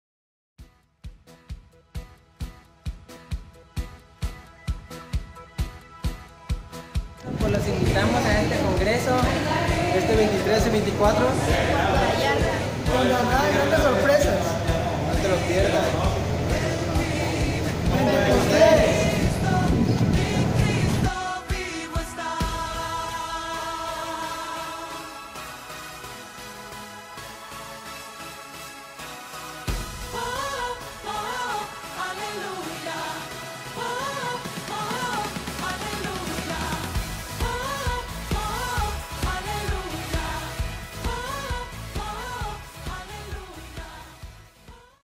PENTECOSTES
JÓVENES VIENE UN GRAN AVIVAMIENTO, NO DESAPROVECHES LA OPORTUNIDAD Y VAMONOS AL CONGRESO PENTECOSTES, LOS ESPERAMOS. SALUDOS Y BENDICIONES